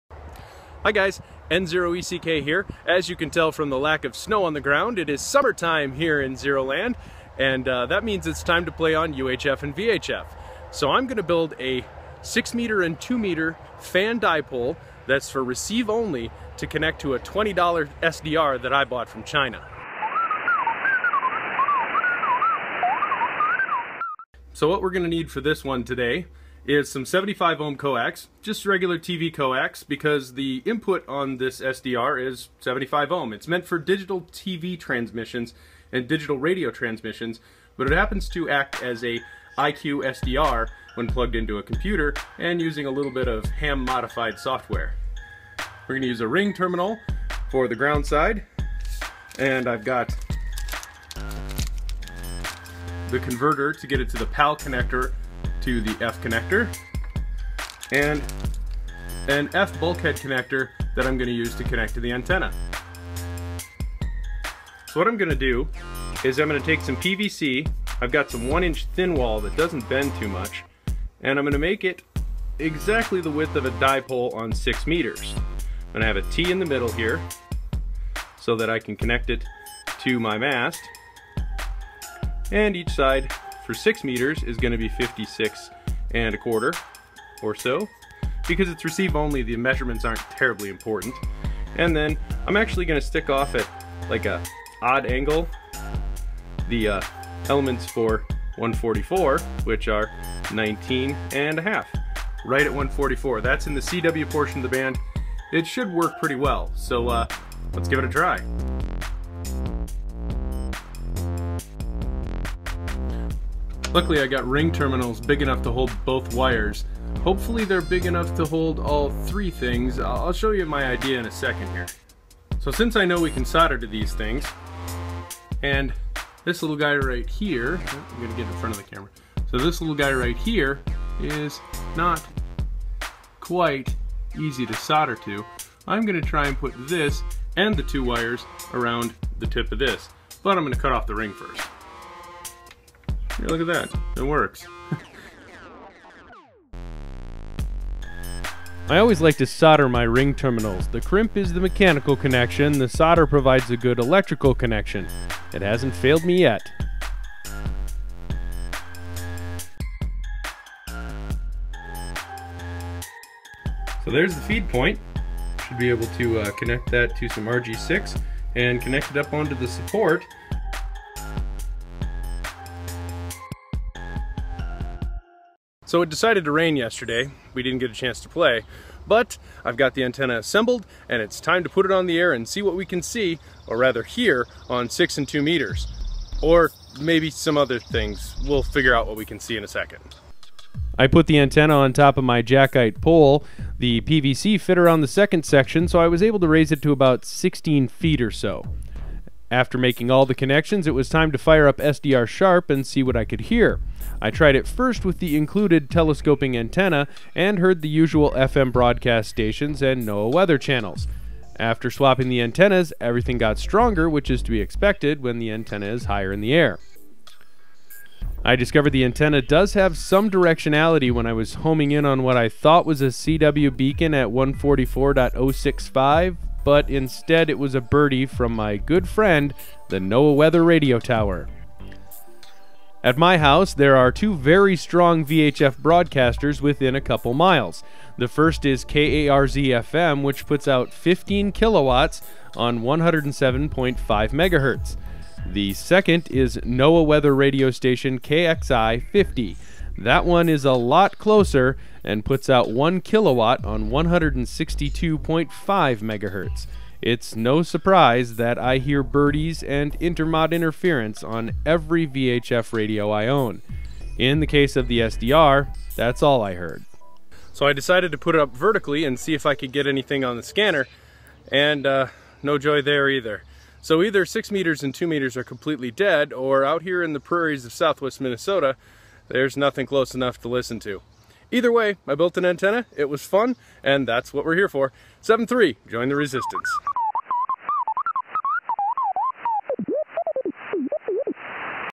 VHF RX Dipole for RTL-SDR
This project is a quick and cheap portable dipole for 6m and 2m. To overcome the cross polarization loss of 30dB I wanted something that was horizontal to listen for SSB and CW signals with my RTL-SDR dongle.